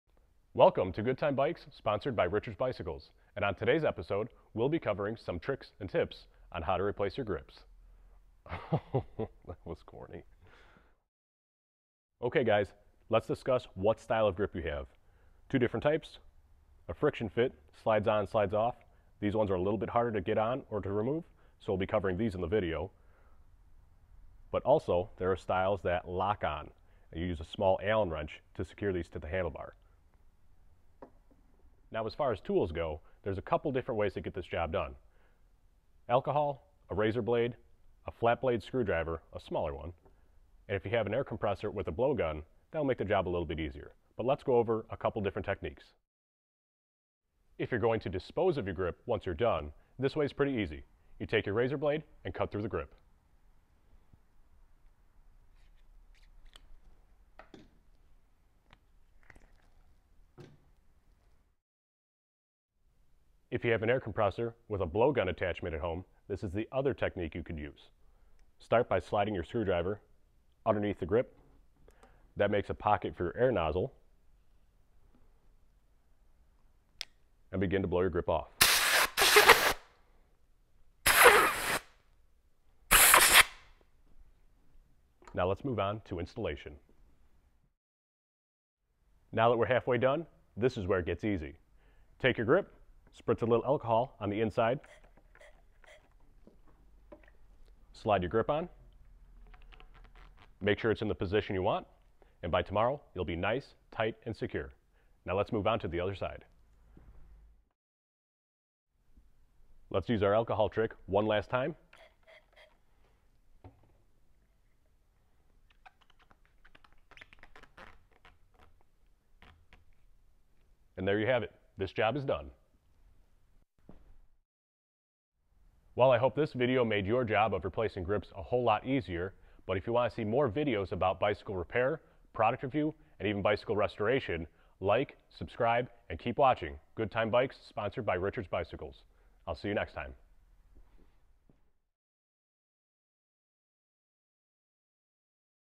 How to Replace Grips on your Bicycle
On this episode of Good Time Bikes I demonstrate a couple different ways to remove your old grips and reveal a helpful trick of how to install your new ones.